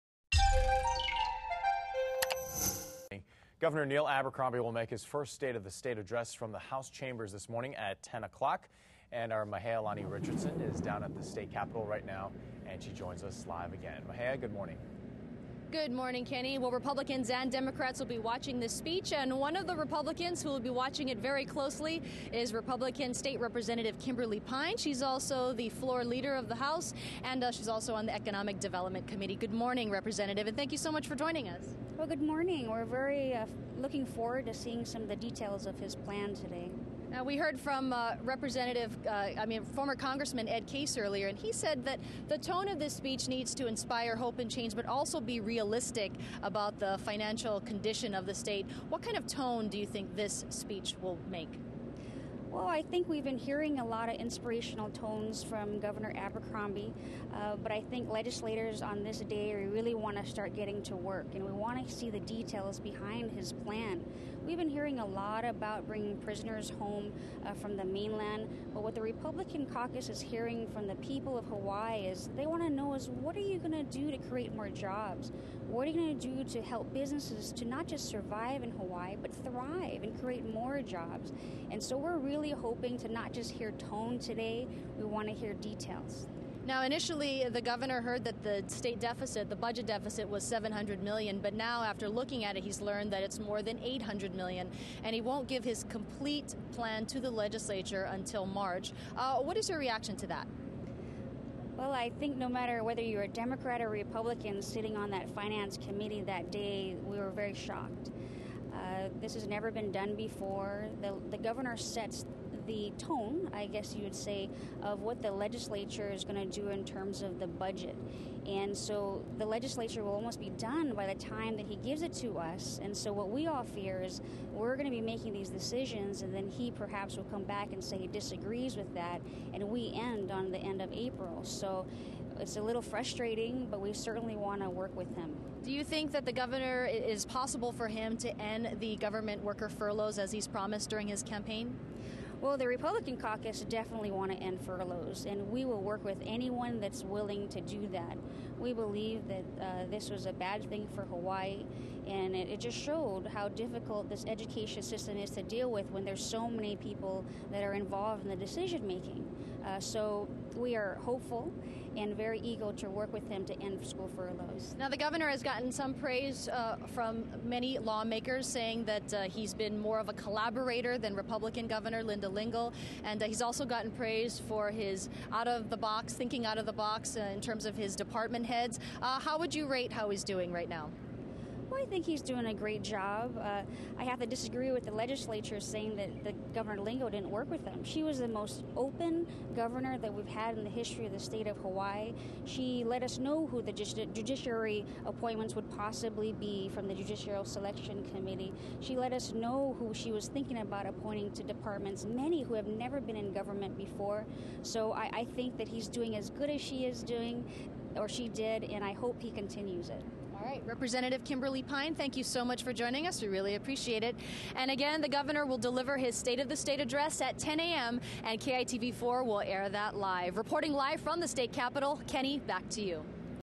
Delivering The Details: Rep. Kymberly Pine
KITV4's Mahealani Richardson talks with Representative Kymberly Pine on Governor Abercrombie's State of the State Address. For news you need to know,